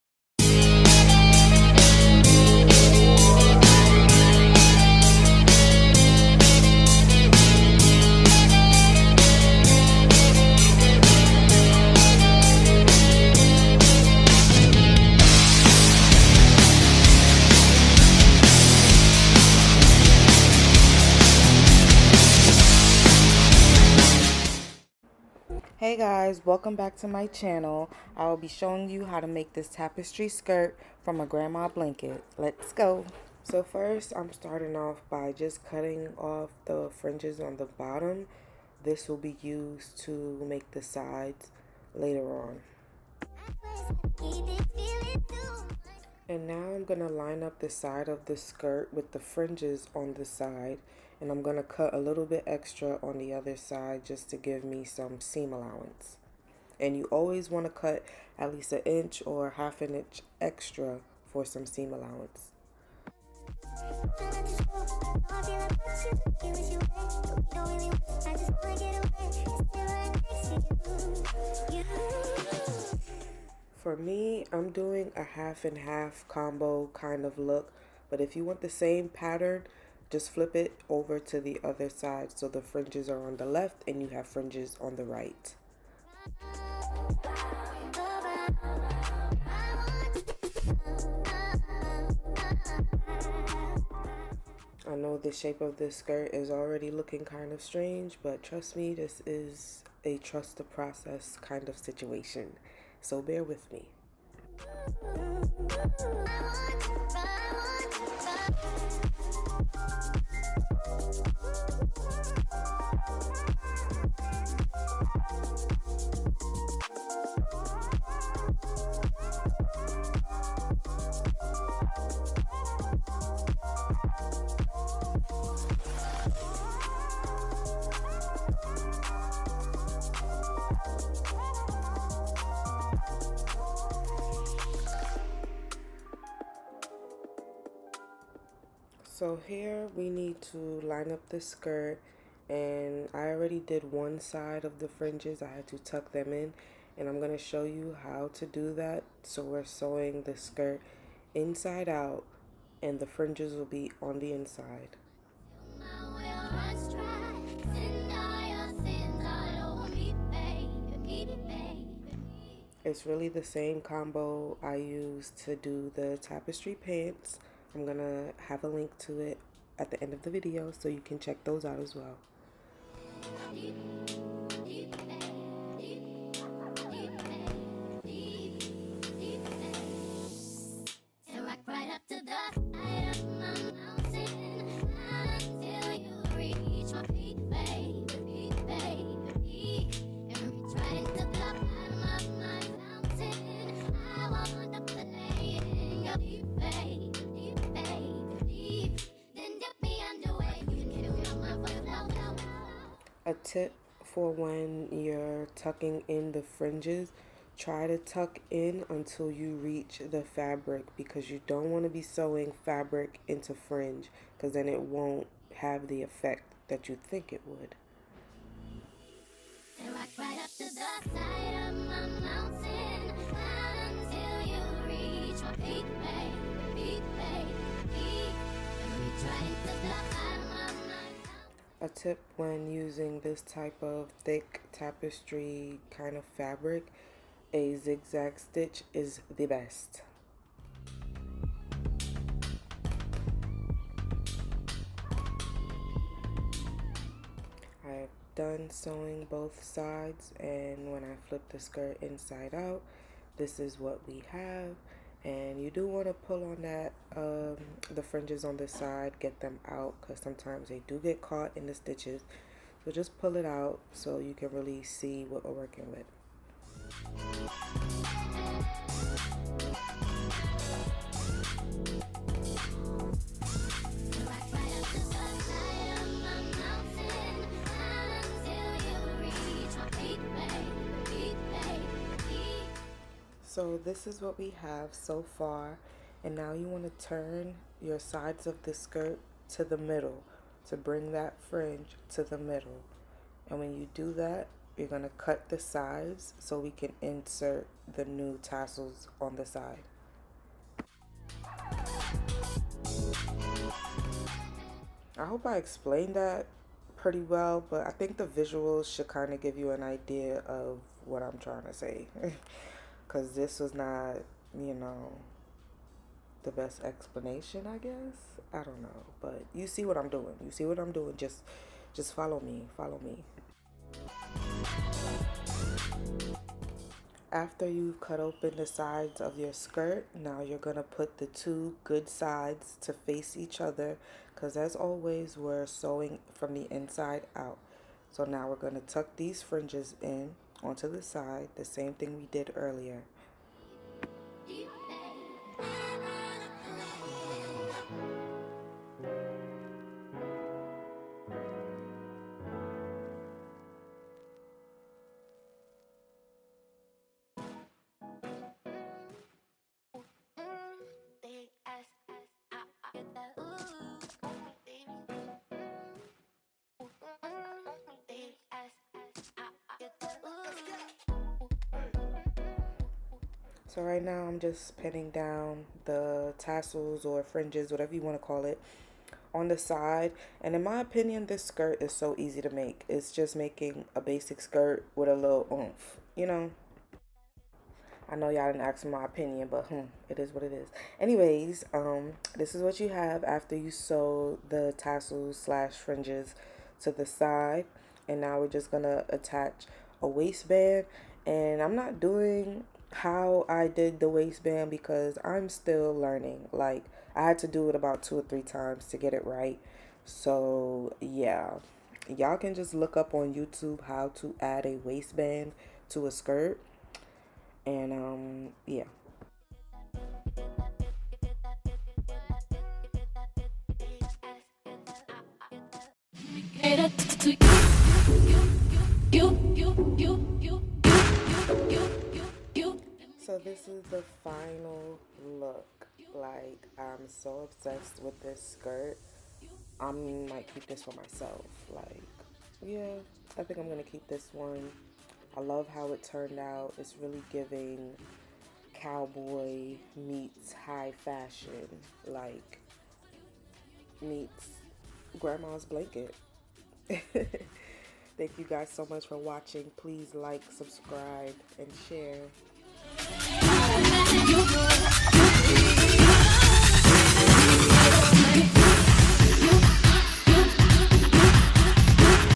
DIY Tapestry Skirt | Blanket Skirt
Watch along as I make this thrifted blanket into this high fashion skirt. Blankets can be found at any thrift stores, I've had most luck with Goodwill. This is an original design so please tag me if you recreate this look ❤️